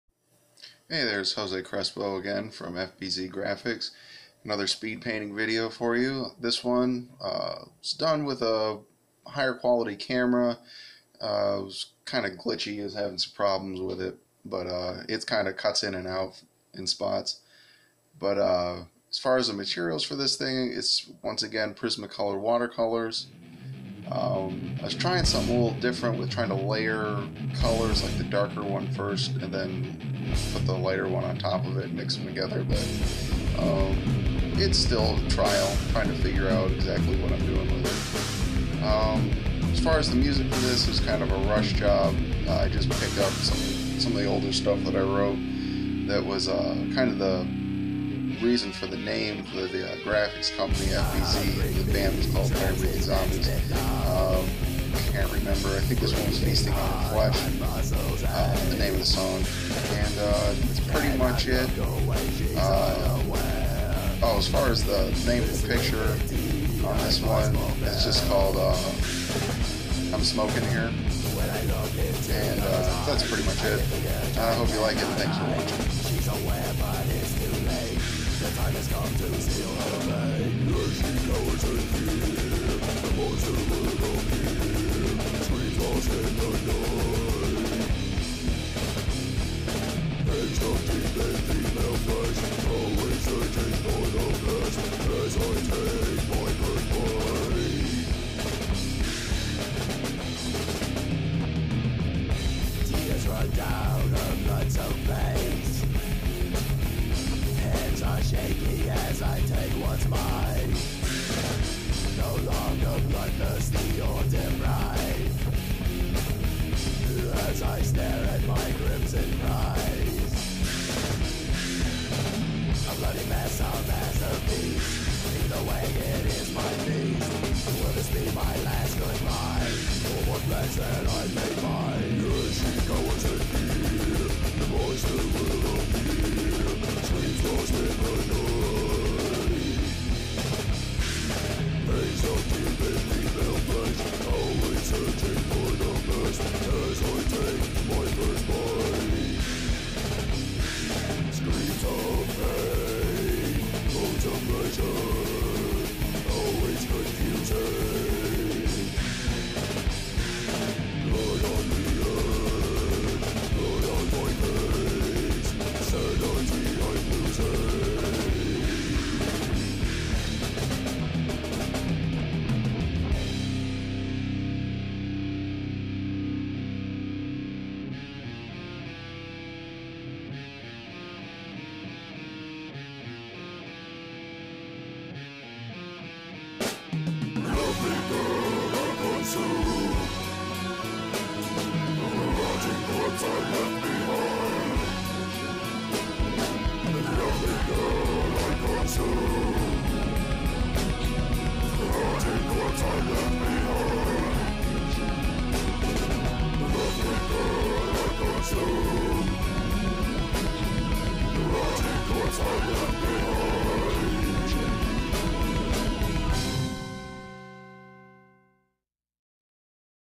I'M SMOKING IN HERE
Speed painting video using Prismacolor watercolor pencils